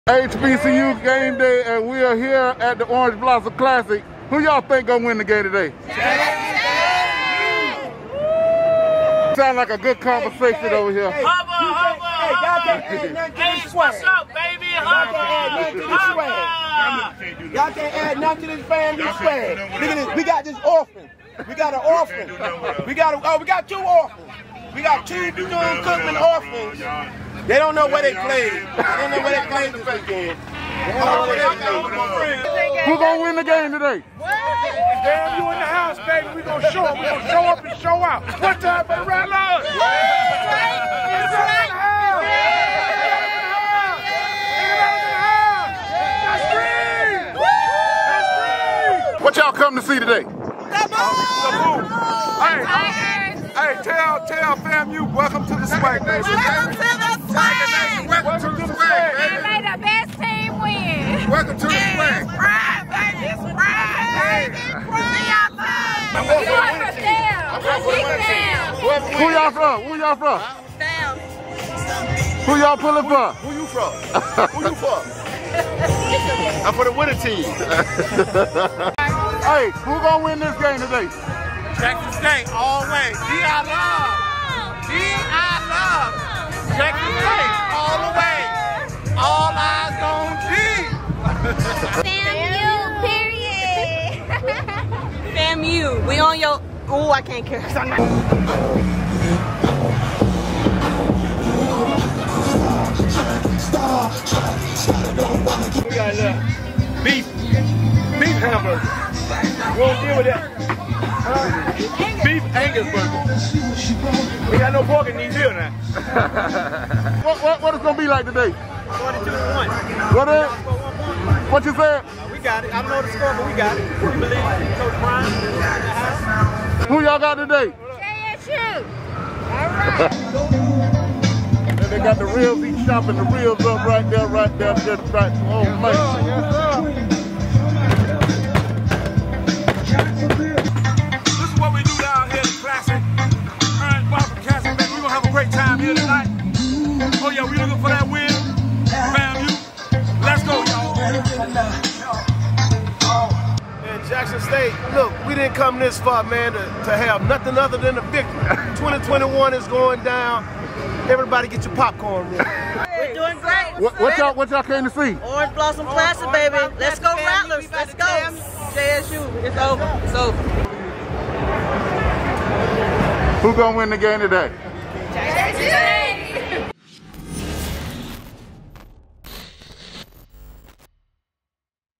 Rattlers and Tigers HYPE at OBC tailgate
We pulled up to the tailgate at the Orange Blossom Classic and the Jackson State and FAMU fans were too HYPE!